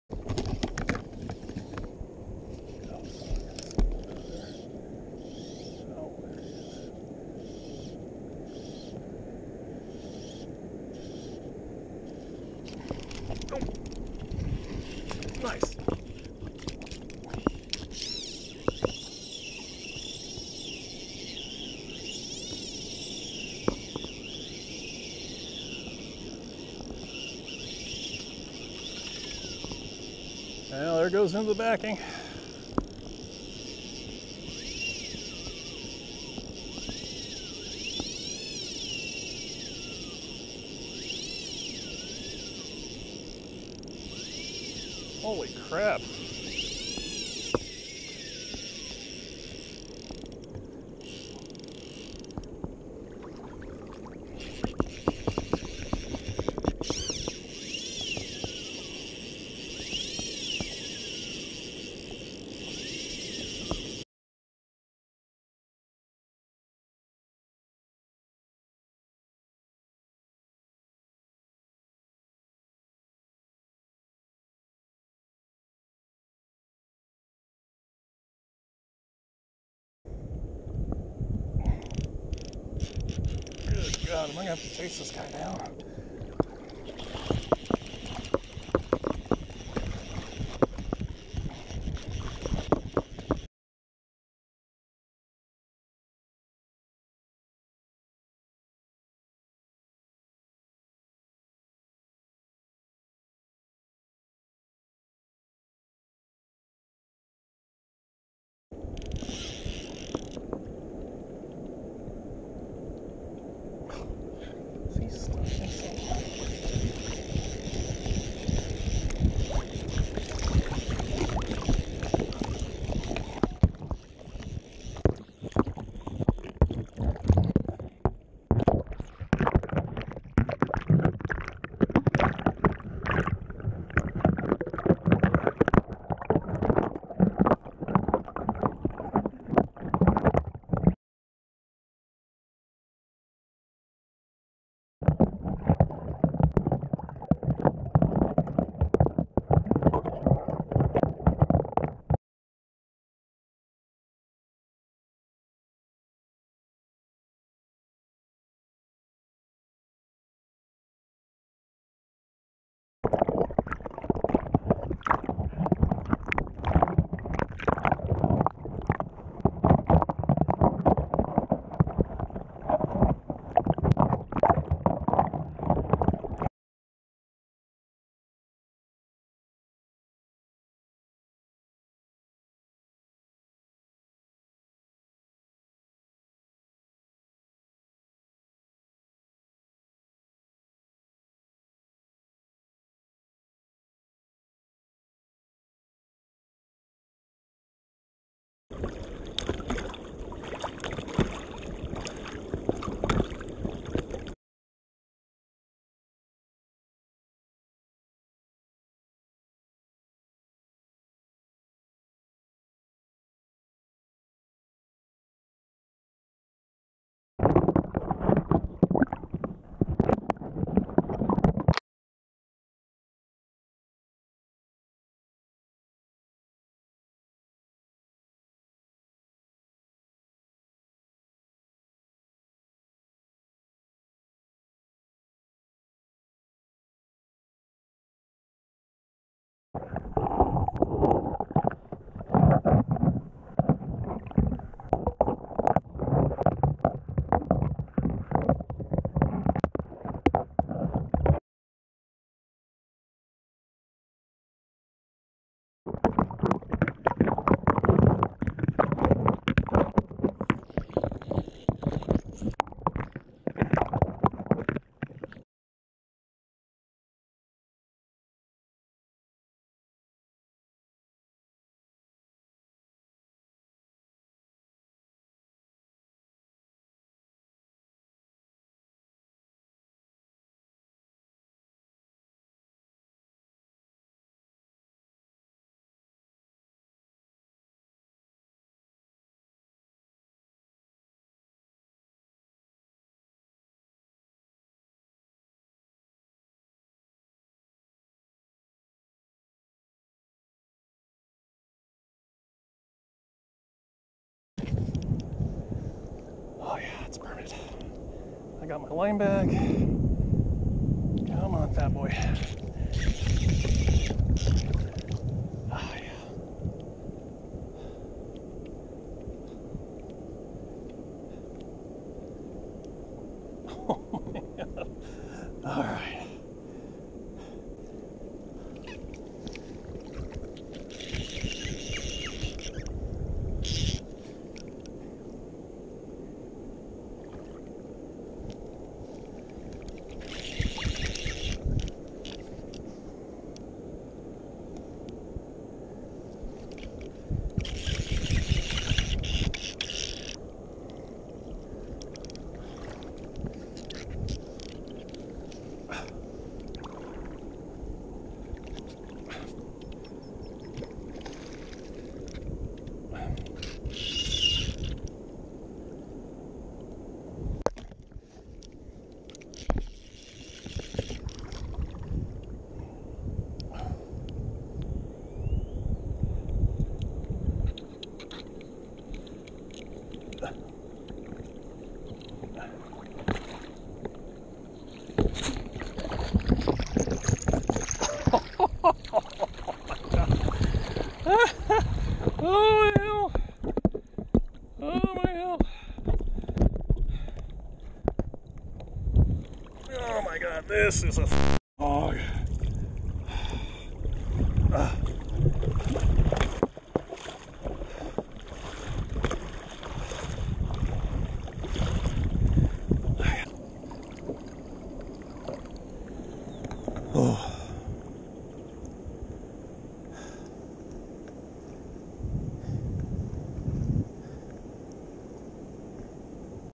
Permit number 24, DIY Saltwater Fly Fishing
Short Video on catching permit number 24 DIY. Includes a couple of swims ;)